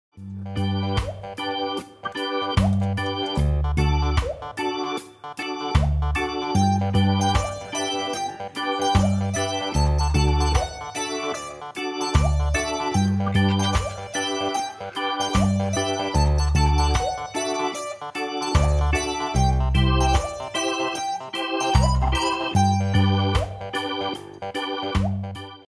장난꾸러기 미어캣
제2아프리카관의 귀염둥이 미어캣
건조하고 탁트인 공간을 좋아하며, 날씨로부터 피해를 막기 위해
굴을 만들어 은신처로 사용한다.